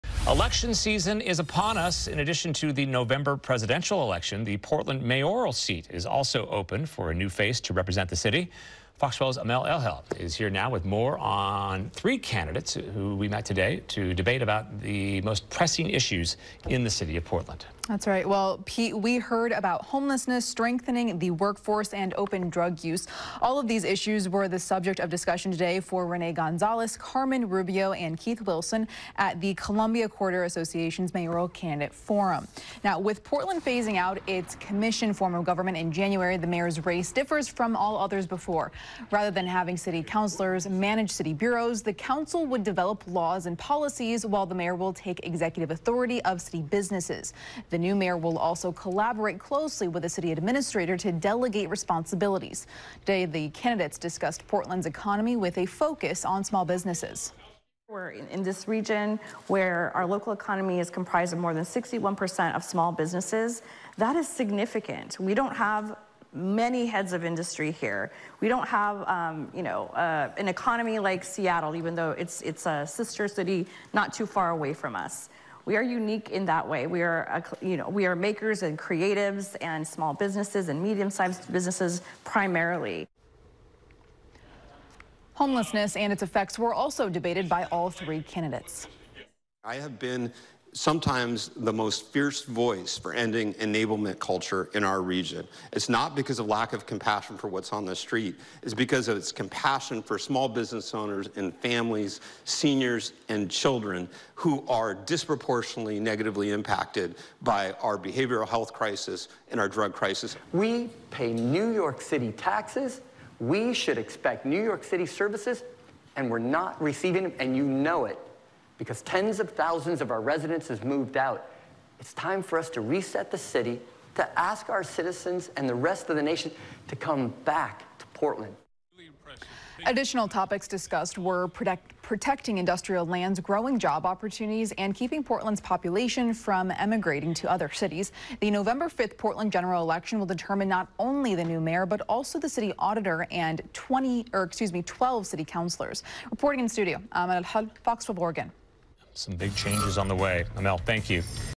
Mayoral candidates Gonzalez, Rubio, Wilson debate Portland issues
Candidates Rene Gonzalez, Carmen Rubio and Keith Wilson met Thursday to debate some of Portland’s most pressing issues.